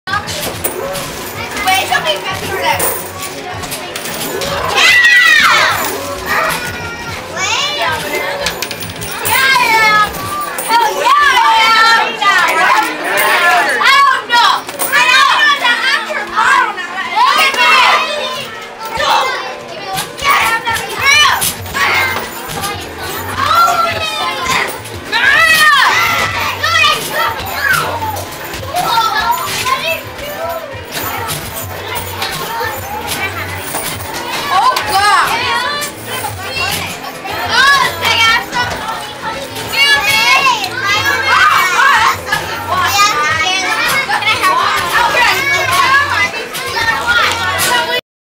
Hannah's Birthday Party
This was Hannah's Birthday Party at CiCi's on Saturday, August 27, 2011